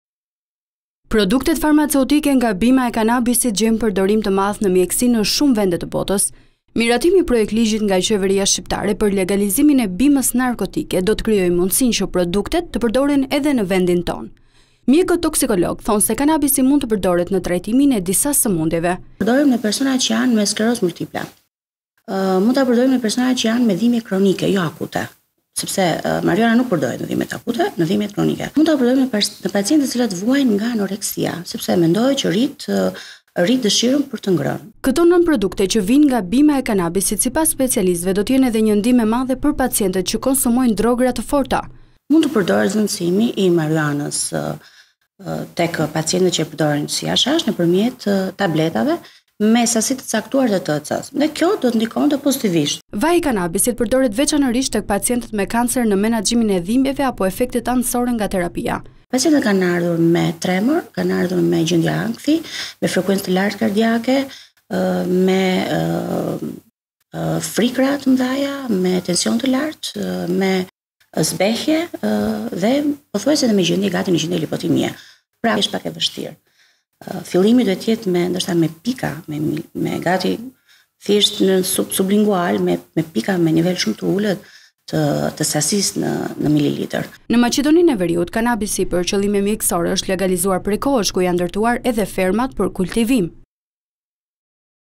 Prodhimi I kanabisit ne mekesi/ Mjeket: Sherben per trajtimin e disa semundjeve - Vizion Plus
Produktet farmaceutike nga bima e kanabisit gjejnë përdorim të madh në mjekësi në shume vende të botës. Miratimi i projektligjit nga qeveria shqiptare për legalizimin e bimës narkotike do të krijojë mundësinë që produktet të përdoren edhe në vendin tonë. Mjekët toksikologë thonë se kanabisi mund të përdoret në trajtimin e disa sëmundjeve.
SUELA XHUFI
MJEKE TOKSIKOLOGE, QSUT
Këto nënprodukte që vijnë nga bima e kanabisit, sipas specialistëve, do të jetë edhe një ndihmë e madhe për pacientët që konsumojnë drogëra të forta.
Vaji i kanabisit përdoret veçanërisht tek pacientët me kancer, në menaxhimin e dhimbjeve apo efekteve anësore nga terapia.
Në Maqedoninë e Veriut kanabisi për qëllime mjekësore është legalizuar prej kohësh, ku janë ndërtuar edhe fermat për kultivim.